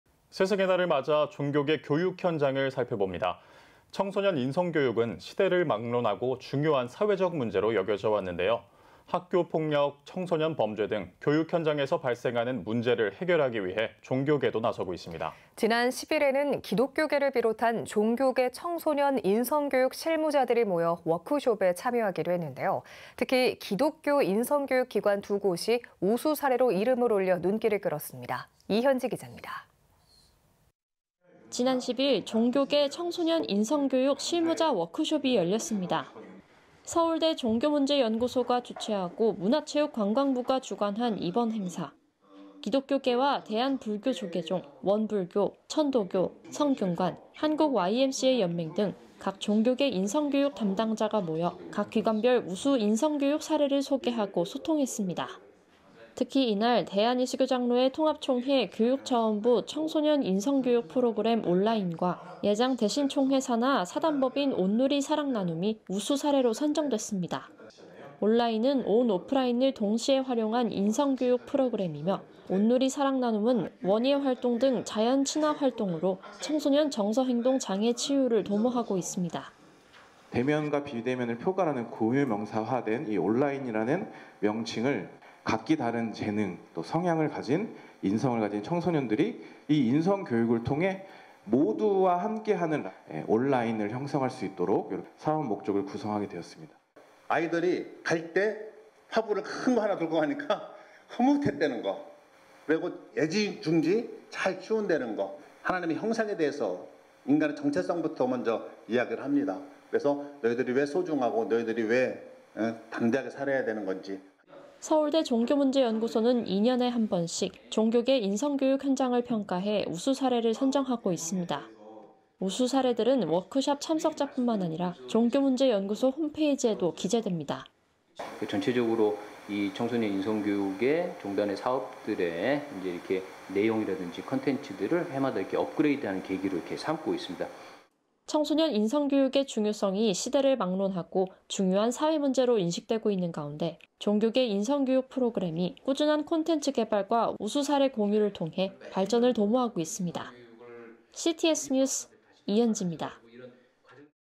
종교계 청소년 인성교육 실무자 모여 ‘워크숍’...기독교 단체 두 곳 우수사례로 선정 (이현지)ㅣCTS뉴스
앵커 : 스승의날을 맞아 종교계 교육 현장을 살펴봅니다. 청소년 인성교육은 시대를 막론하고 중요한 사회적 문제로 여겨져 왔는데요. 학교폭력, 청소년 범죄 등 교육현장에서 발생하는 문제를 해결하기 위해서 종교계도 나서고 있습니다.

앵커 : 지난 10일에는 기독교계를 비롯한 종교계 청소년 인성교육 실무자들이 모여 워크숍에 참여했습니다. 특히 기독교 인성교육 기관 두 곳이 우수사례로 이름을 올려 눈길을 끌었습니다. 이현지 기자입니다.

지난 10일 종교계 청소년 인성교육 실무자 워크숍이 열렸습니다.

서울대 종교문제연구소가 주최하고 문화체육관광부가 주관한 이번 행사.

기독교계와 대한불교조계종, 원불교, 천도교, 성균관, 한국YMCA연맹 등 각 종교계 인성교육 담당자가 모여 각 기관 별 우수 인성교육 사례를 소개하고 소통했습니다.

특히 이날 대한예수교장로회(통합) 총회 교육자원부 청소년 인성교육 프로그램 ‘올라인’과 예장(대신) 총회 산하 ‘(사)온누리사랑나눔’이 우수사례로 선정됐습니다.

‘올라인’은 온오프라인을 동시에 활용한 인성교육 프로그램이며, ‘온누리사랑나눔’은 원에활동 등 자연친화 활동으로 청소년 정서행동 장애 치유를 도모하고 있습니다.

[우진하 목사 / 예장(통합) 총회 교육자원부 ‘올라인’ 담당자]
대면과 비대면을 포괄하는 고유 명사화된 올라인이라는 명칭으로 각기 다른 재능 또 성향을 가진 인성을 가진 청소년들이 이 인성 교육을 통해 모두와 함께하는 올-라인을 형성할 수 있도록사업 목적을 구성하게 되었습니다

[전병재 이사장 / 예장(대신) 총회 (사)온누리사랑나눔]
(인성교육 대상) 아이들이 갈 때 화분을 큰 거 하나 들고 가니까 흐뭇했다는 거 그리고 자기 애지중지 잘 키운다는 거 저는 (창조주) 하나님의 형상에 대해서 인간의 정체성부터 먼저 이야기를 합니다 너희들이 왜 소중하고 너희들이 왜 담대하게 살아야 되는 건지

서울대 종교문제연구소는 2년에 한 번씩 종교계 인성교육 현장을 평가해 우수사례를 선정하고 있습니다.

우수 사례들은 워크샵 참석자뿐만 아니라 종교문제연구소 홈페이지에도 공유됩니다.

[성혜영 교수 / 서울대 종교학과]
전체적으로 이 청소년 인성교육의 종단의 사업들의 이제 이렇게 내용이라든지 콘텐츠들을 해마다 이렇게 업그레이드하는 계기로 이렇게 삼고 있습니다

청소년 인성교육의 중요성이 시대를 막론하고 중요한 사회 문제로 인식되고 있는 가운데, 종교계 인성 교육 프로그램이 꾸준한 콘텐츠 개발과 우수사례 공유를 통해 발전을 도모하고 있습니다. CTS 뉴스 이현지입니다.